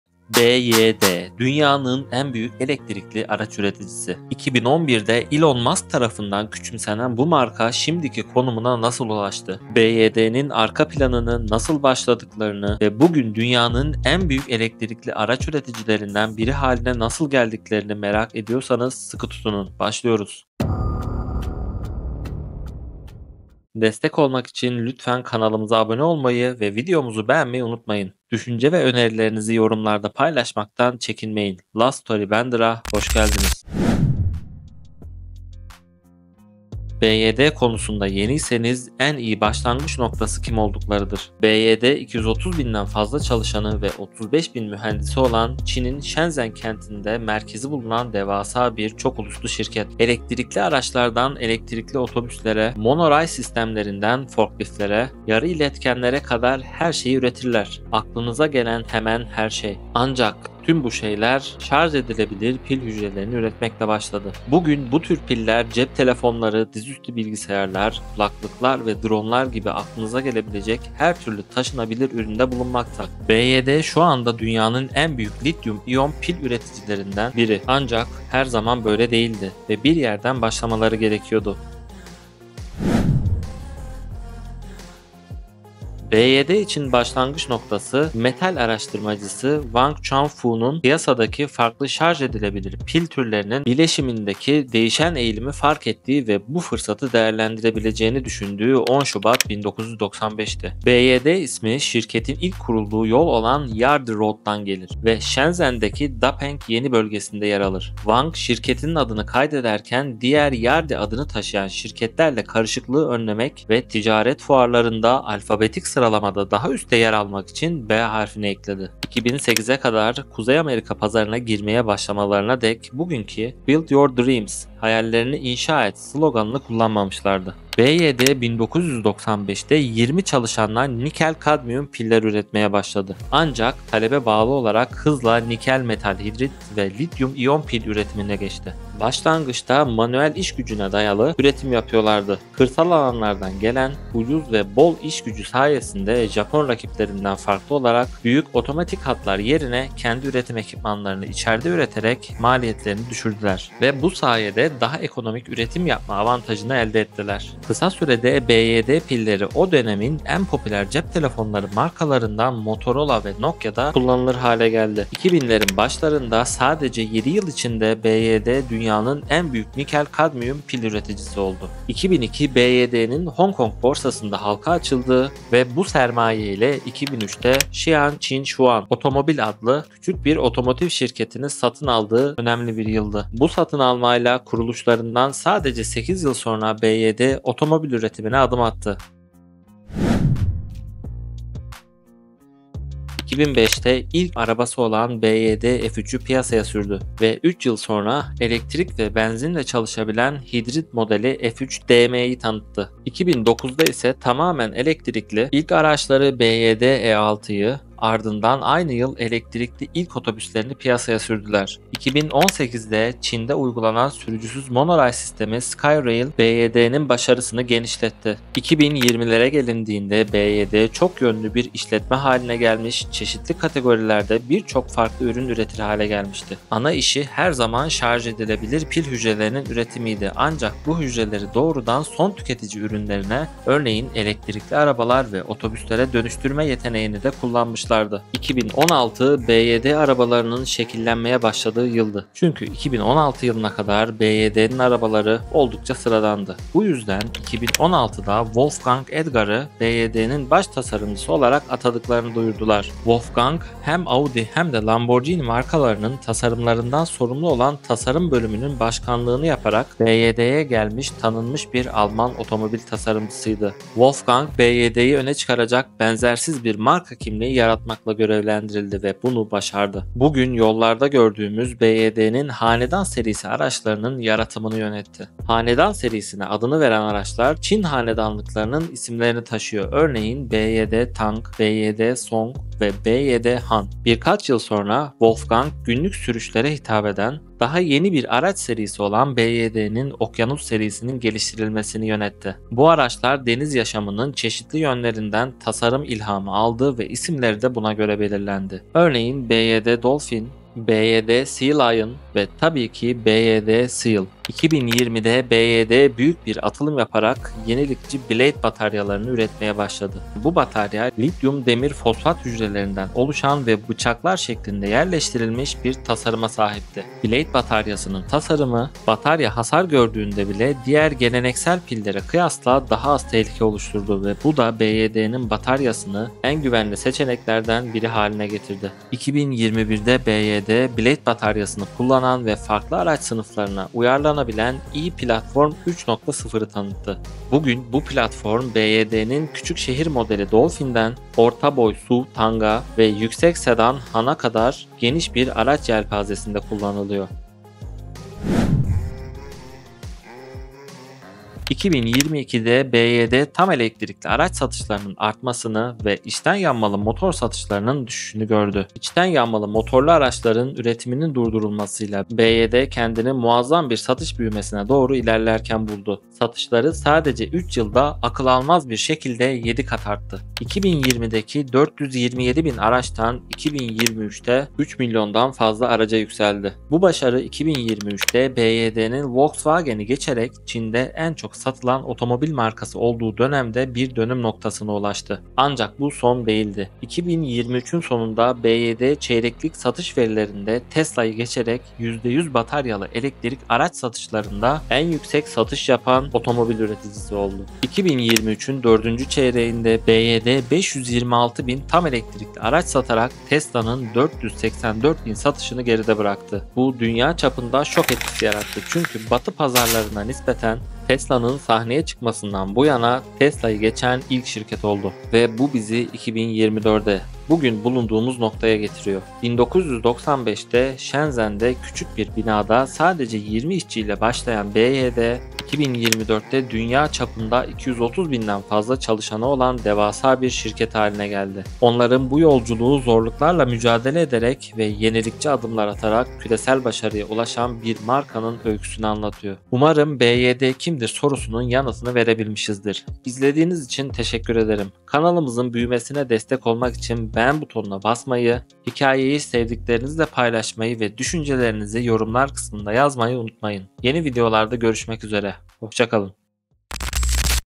Tesla’nın Kabusu Olan Çinli Şirket: BYD | BYD Başarı Hikayesi
BYD. Dünyanın en büyük elektrikli araç üreticisi! 2011 de Elon Musk tarafından küçümsenen bu marka bu konuma nasıl ulaştılar? BYD’ nin arka planını, nasıl başladıklarını ve bugün dünyanın en büyük elektrikli araç üreticilerinden biri haline nasıl geldiklerini merak ediyorsanız sıkı tutunun başlıyoruz. BYD konusunda yeniyseniz , en iyi başlangıç noktası kim olduklarıdır. BYD, 230.000'den fazla çalışanı ve 35.000 mühendisi olan, Çin'in Shenzhen kentinde merkezi bulunan devasa bir çok uluslu şirkettir. Elektrikli araçlardan elektrikli otobüslere, monoray sistemlerinden forkliftlere, yarı iletkenlere kadar her şeyi üretirler, aklınıza gelen hemen her şey. Ancak her şey, bu şarj edilebilir pil hücrelerini üretmekle başladı. Bugün, bu tür piller, cep telefonları, dizüstü bilgisayarlar, kulaklıklar ve drone' lar gibi aklınıza gelebilecek her türlü taşınabilir üründe bulunmakta.
Düşüncelerinizi ve önerilerinizi yorumlarda paylaşmaktan çekinmeyin.